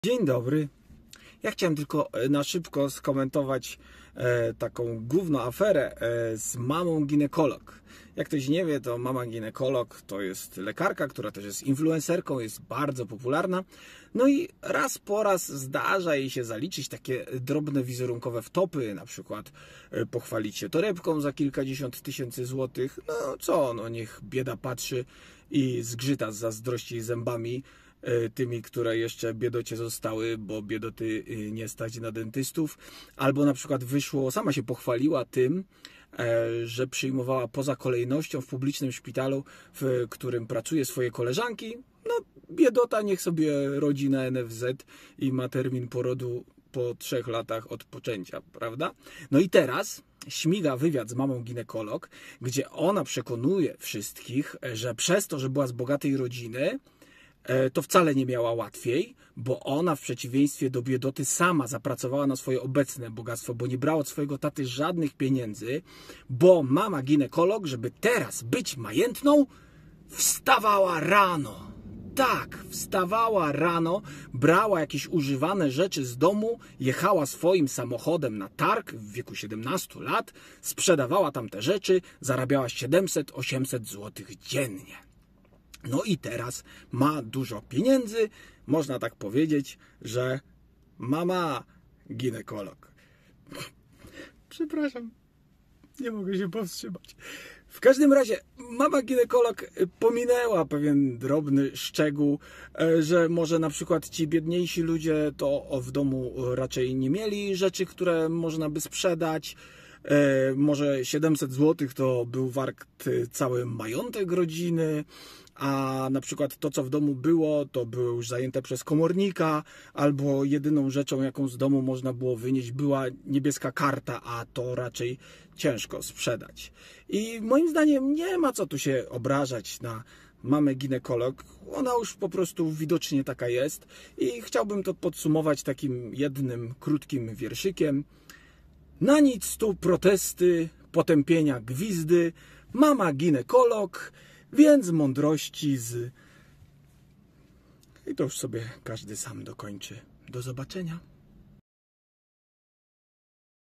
Tomek Nowaczyk - "Afera" z Mamą Ginekolog
W komentarzach otrzymuję pytania, czemu nie zajmuję się poważnymi rzeczami. A bo ja biorę na tapet tylko gó*noafery. Dziś Mama Ginekolog.